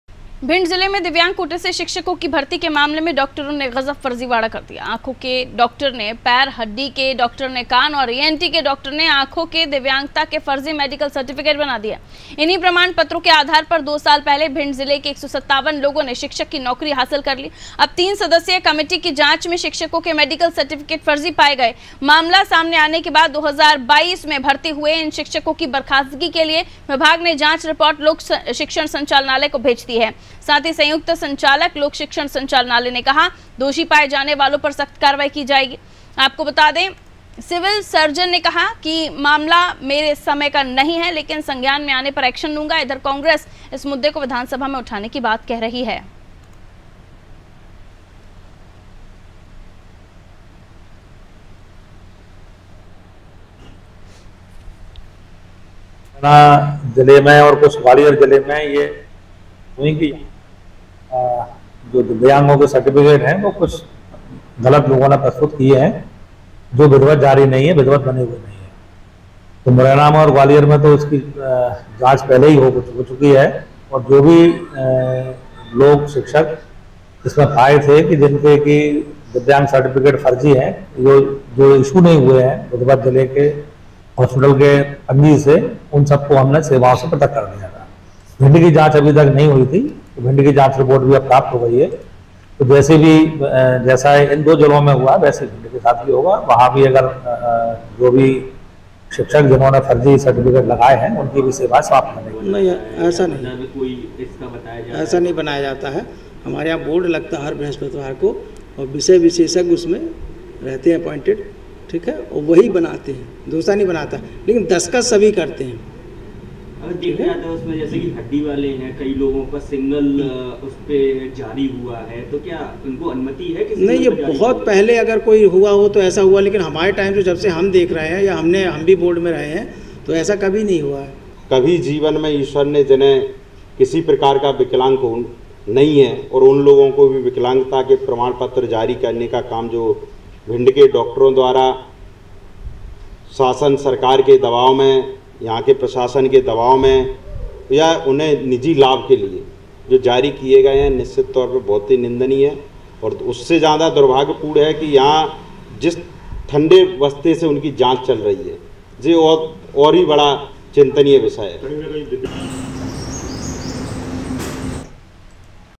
Bhind : MP अजब.. फर्जीवाड़ा गजब !, फर्जी दिव्यांगता प्रमाण-पत्र से बने शिक्षक || Anaadi Tv
AIRTEL - 379